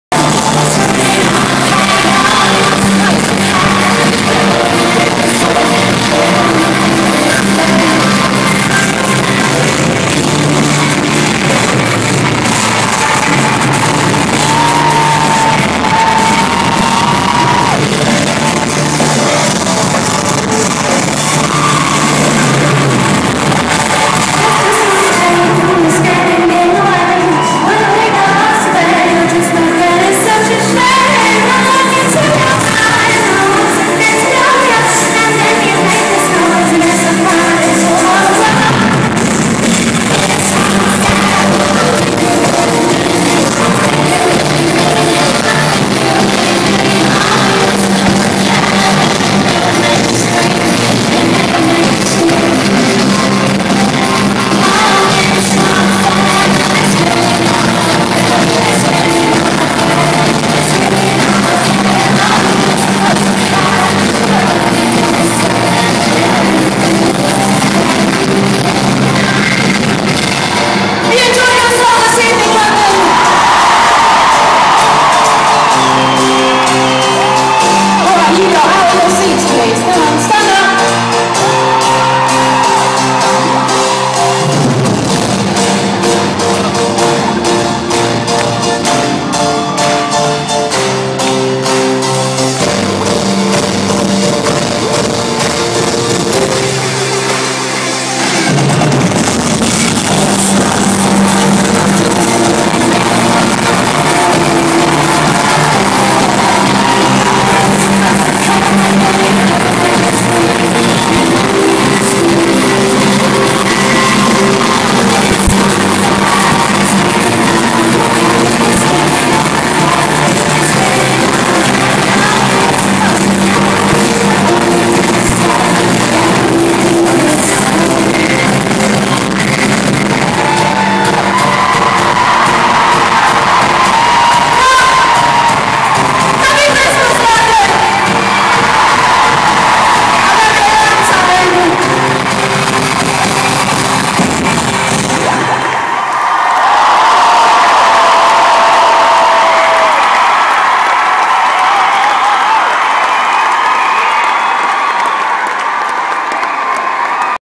Lily Allen at Brixton Academy
From the 2009 December gig in Brixton, London